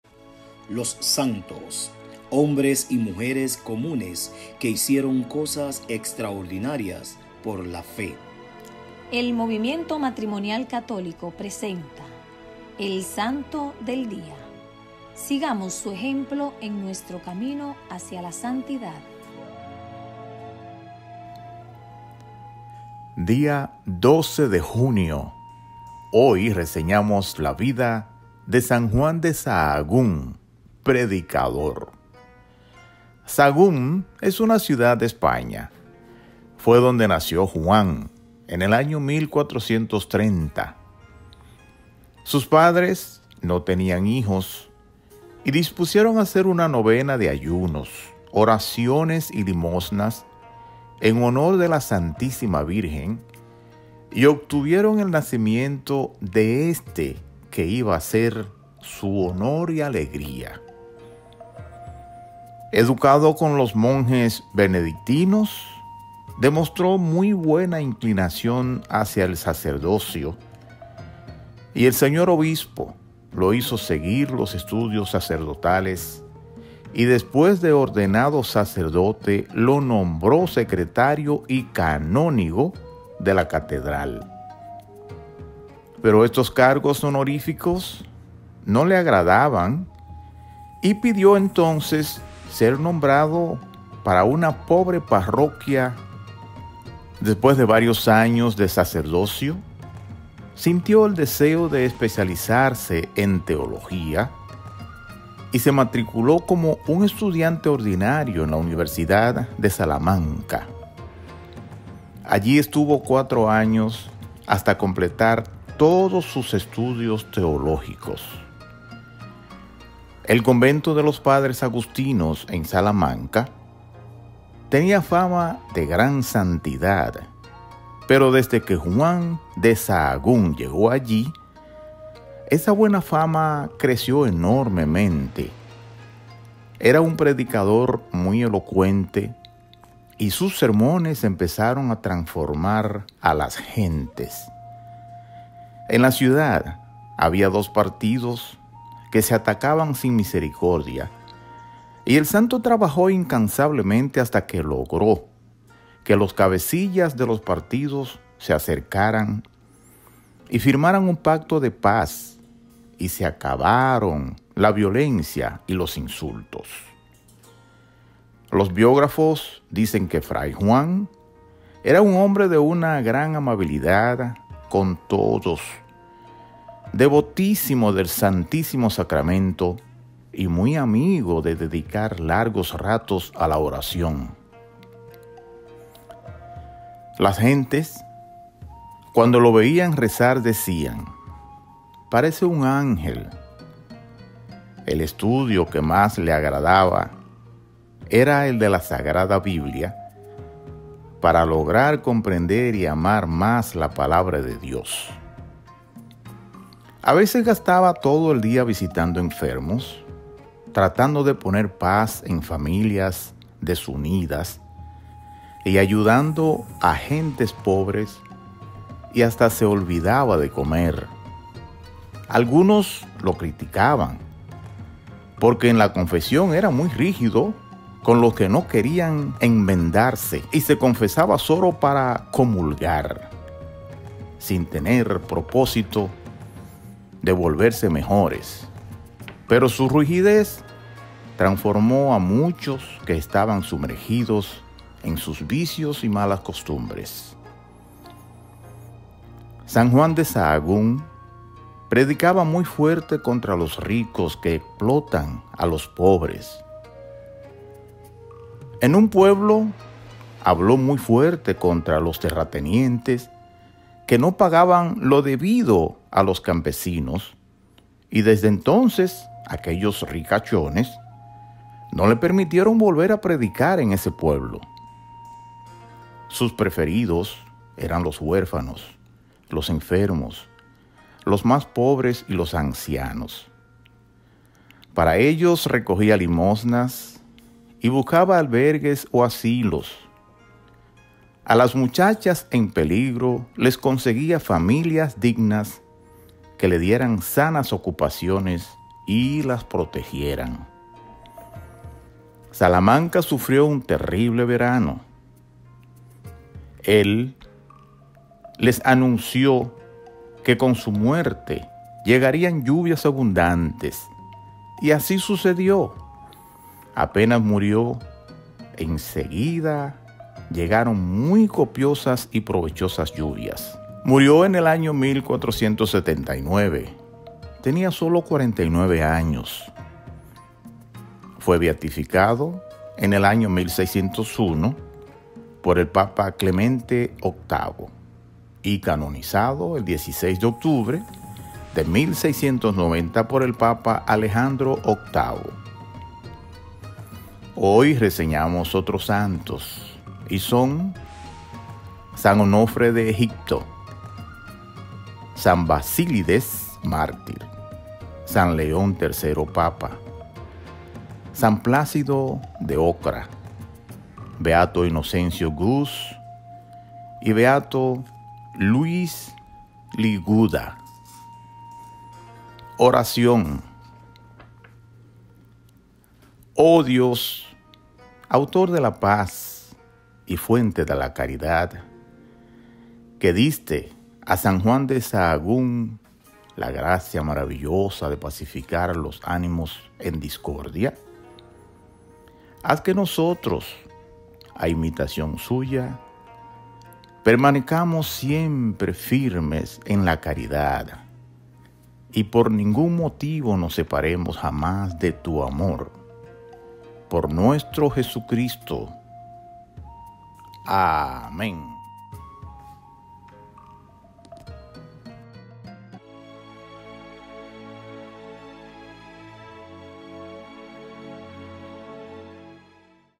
MMC SANTO DEL DIA 12 DE JUNIO - SAN JUAN DE SAHAGÚN
1- Que mensaje te deja personalmente el santo del día de hoy?

2- Cuál es la virtud de este santo que quieres imitar en tu vida

OTROS SANTOS DEL DIA DE HOY SON
San Onofre de Egipto
San Basílides mártir
San León III papa
San Plácido de Ocra
Beato Inocencio Guz
Beato Luis Liguda